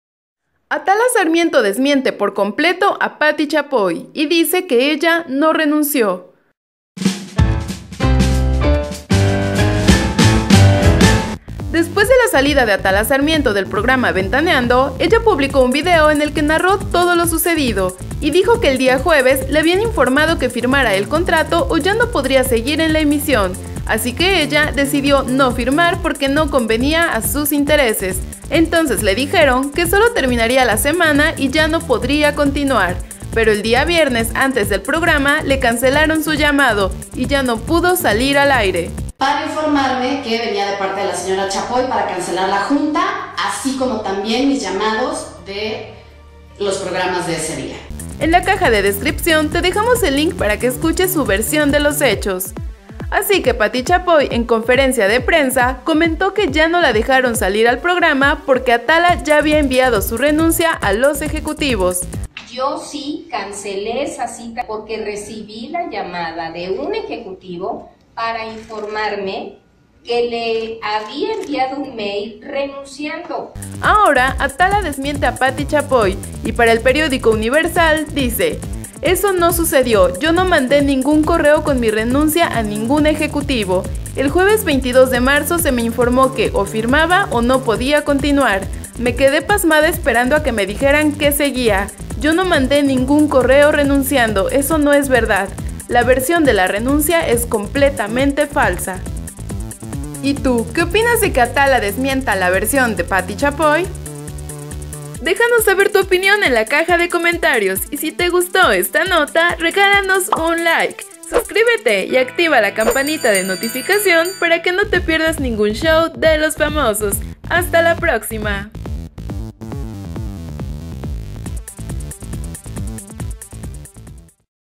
Atala Sarmiento LLAMA MENTIROSA a Pati Chapoy: YO NO RENUNCIÉ a Ventaneando
Atala Sarmiento se lanza contra Pati Chapoy y la llama mentirosa pues dice que ella jamás renunció a Ventaneando.